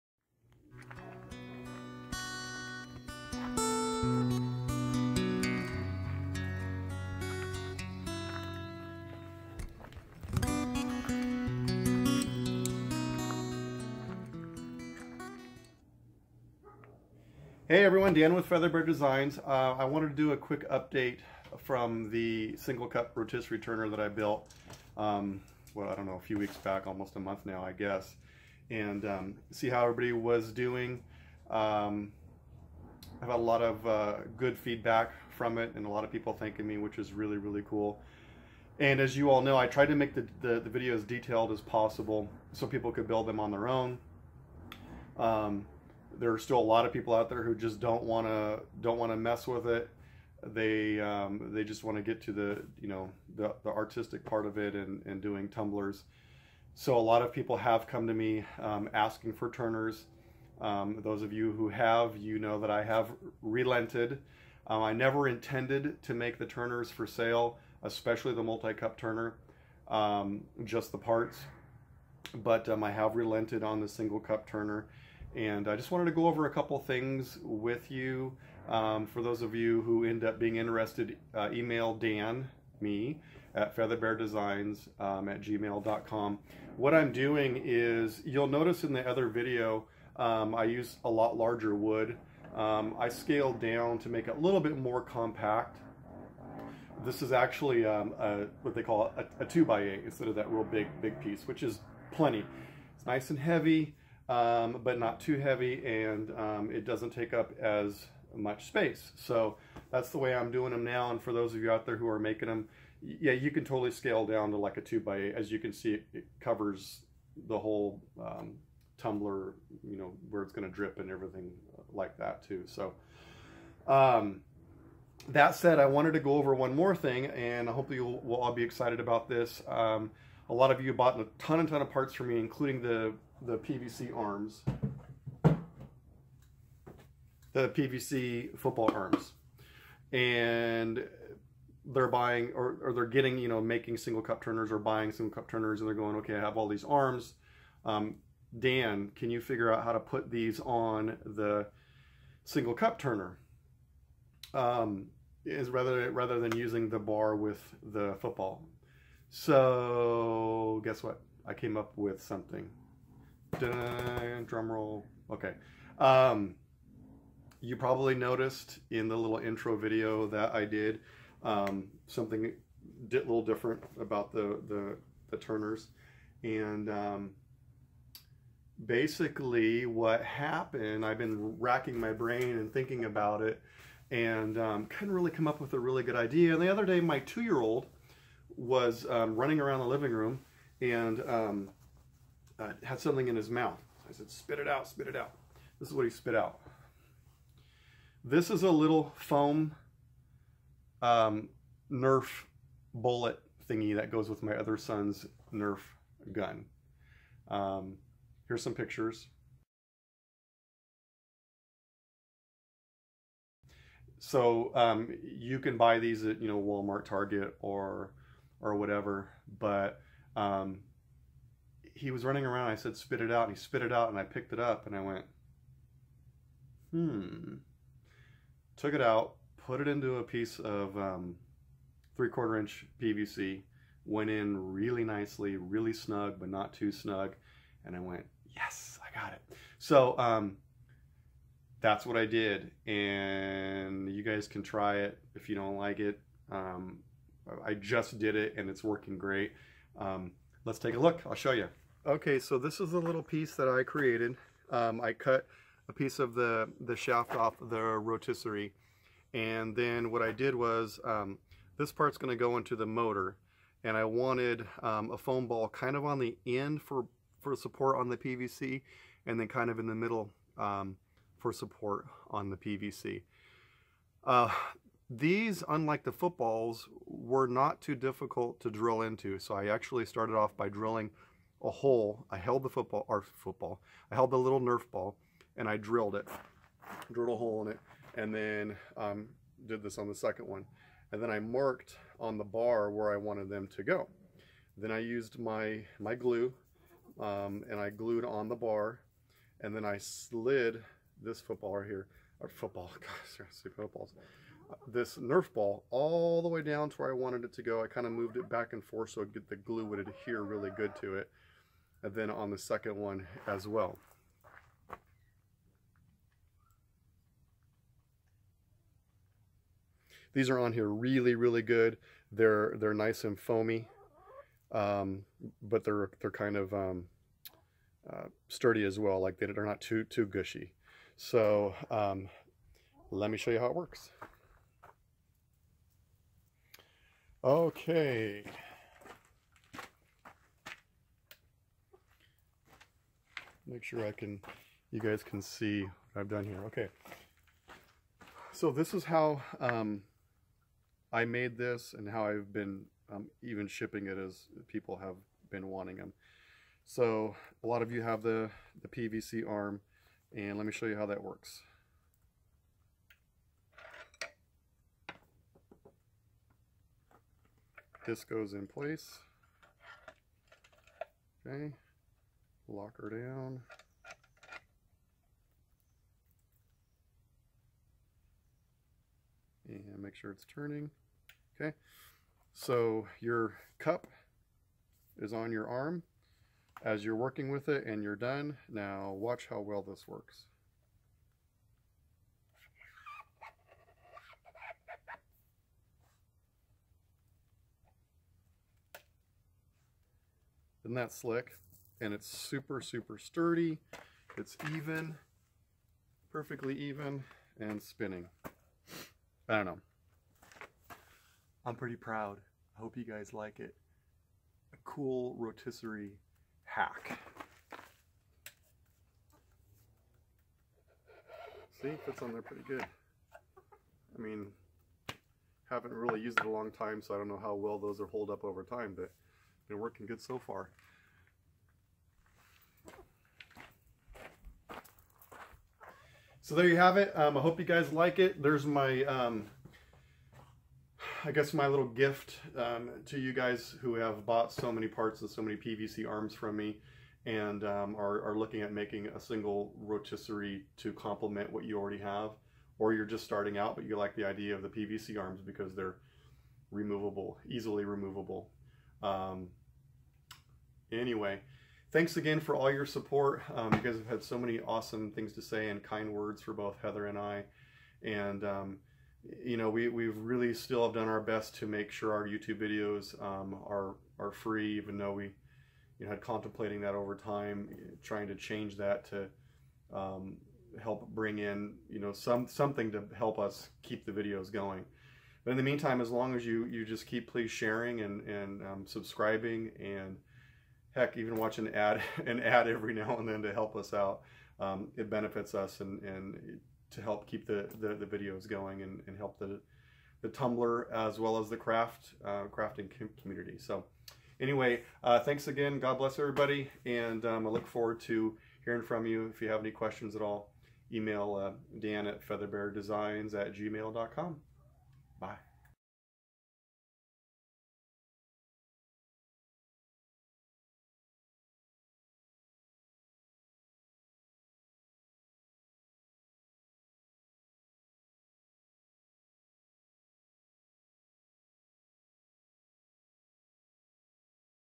Single Rotisserie Tumbler Turner with YOUR PVC football arm!!
Ok, You asked for it.  I think I came up with a way to attach all those PVC football arms out there in the world to the Single Rotisserie Turner bar.  Im sorry parts of the beginning of this were blurry.  My camera was being weird.  Its only for a little in the beginning.